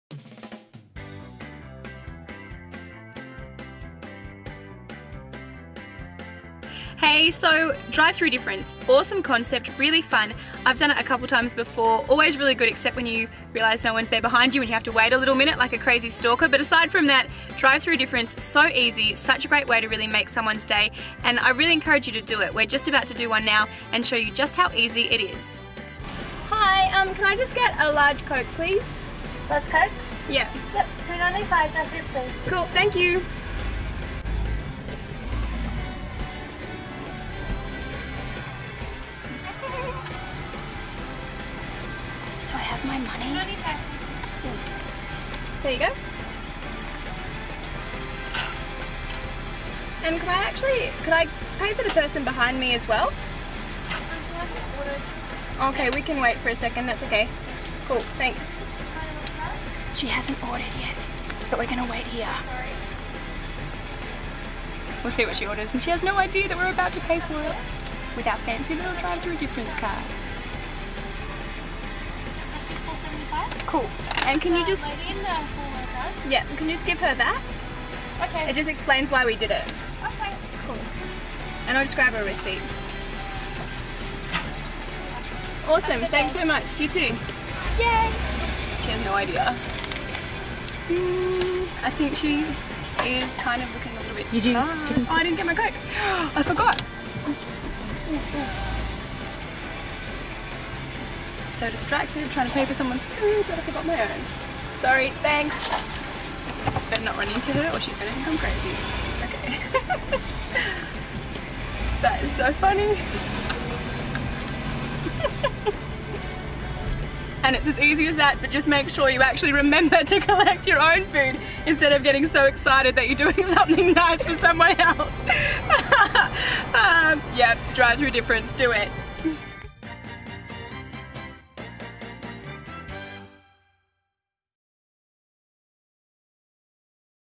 The Drive Thru Difference with Laura Bennett - Hope 103.2
Nights Announcer Laura Bennett gives the 'Drive Thru Difference' a go and demonstrates how you can also give it a go.